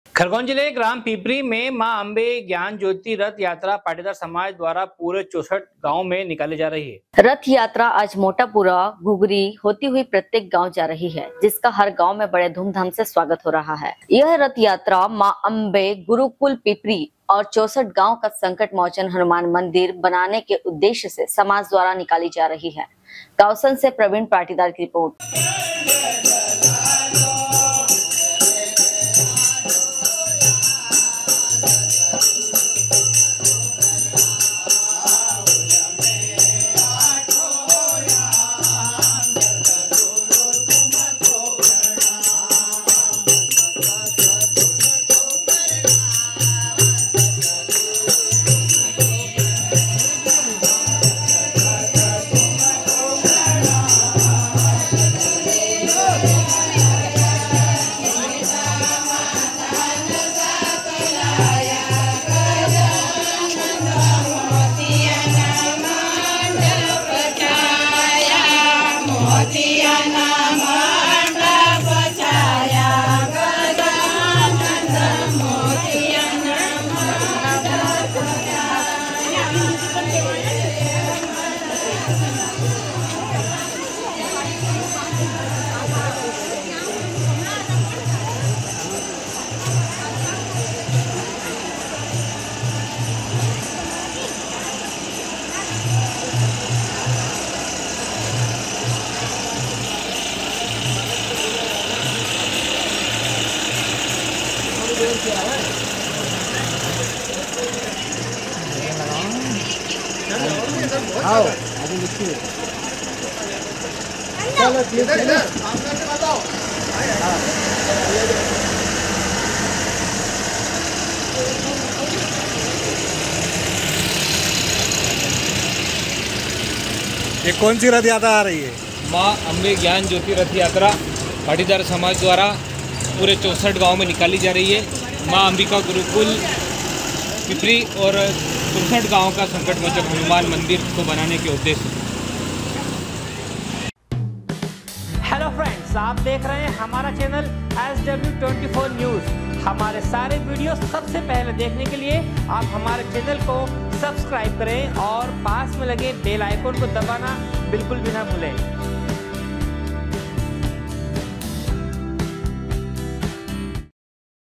जिले के ग्राम पिपरी में मां अंबे ज्ञान ज्योति रथ यात्रा पाटीदार समाज द्वारा पूरे 64 गांव में निकाली
खरगोन जिले के ग्राम पिपरी में मां अंबे ज्ञान ज्योति रथ यात्रा पाटीदार समाज द्वारा पूरे 64 गांव में निकाली जा रही है
गावसन से प्रवीण पाटीदार की रिपोर्ट।